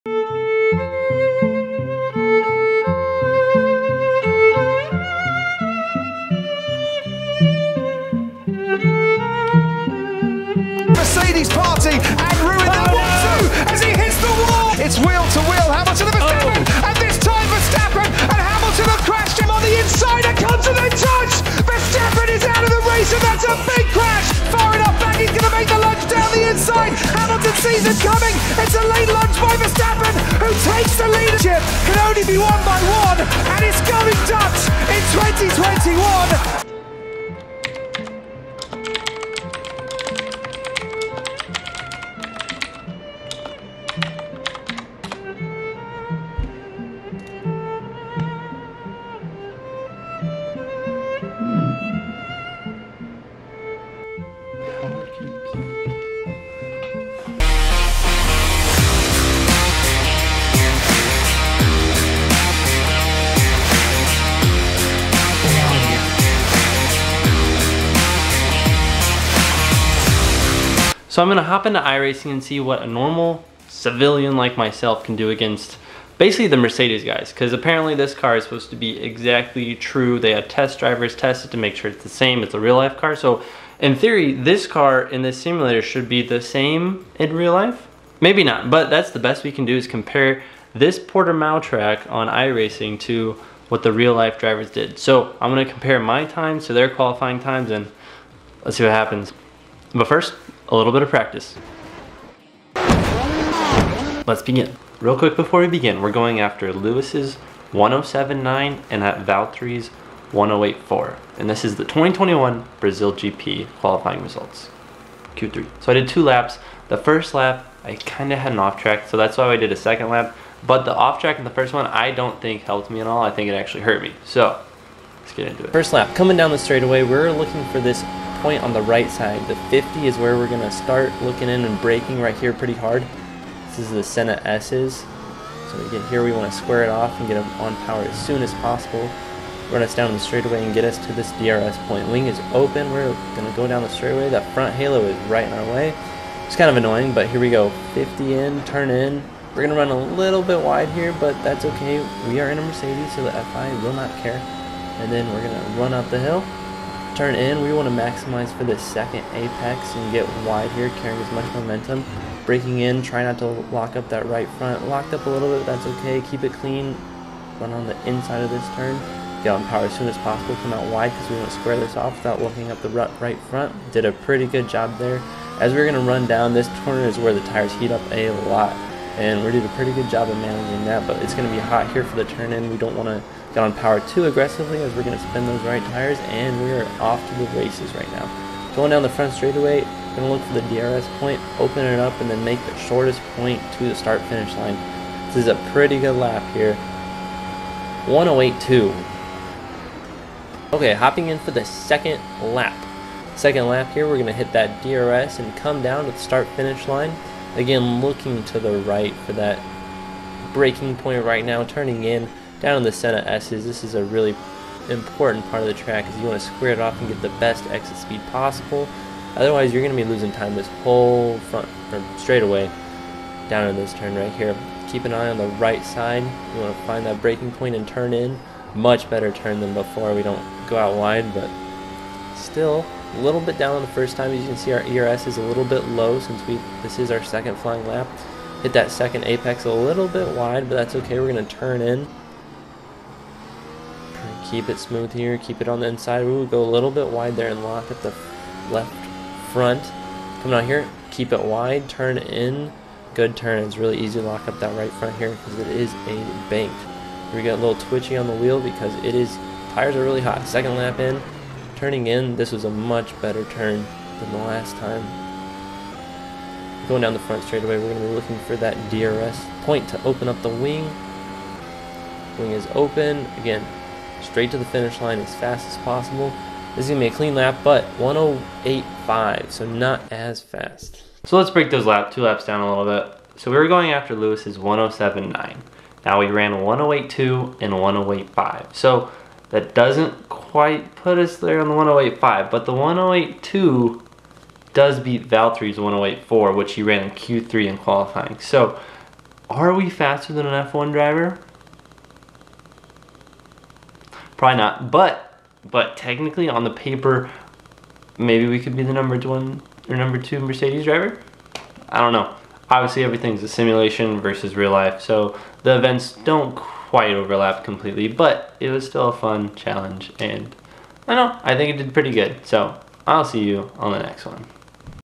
Civilian Vs F1 Driver
Challenging myself to beat the Mercedes 2021 Qualifying Lap in Brazil.